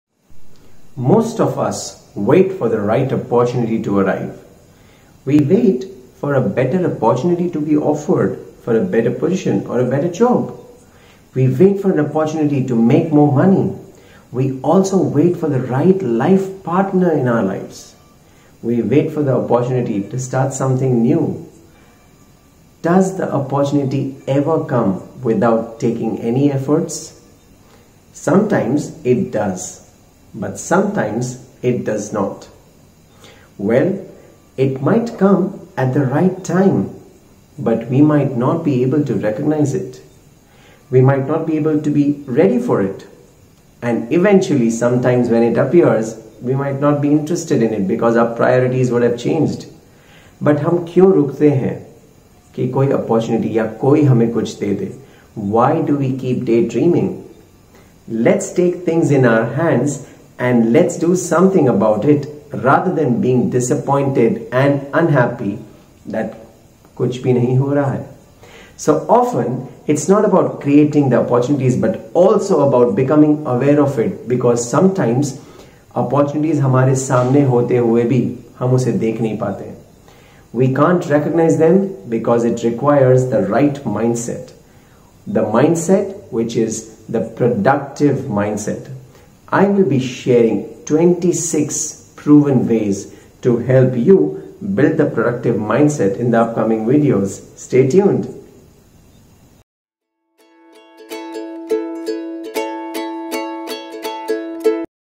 Create opportunity | Never wait for opportunity
Don't wait for the right opportunity to Arrive

Sometimes we wait for a better job
Sometimes we wait for the money
Sometimes we wait for a life partner
Sometimes we wait for the right time to grab opportunities

Opportunities never come without taking any effort.

If you want something, just take things in your hand, and do something about it.